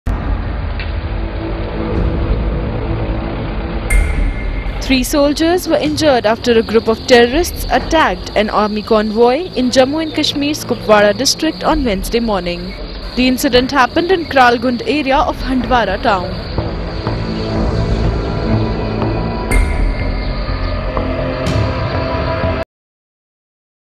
जम्मू कश्मीर में आतंकवादी हमला I Terrorist attack army convoy, three soldiers injured
सेना के काफिले पर हमला, तीन सैनिक जख्मी
बुधवार सुबह कुपवाड़ा जिले में हुआ हमला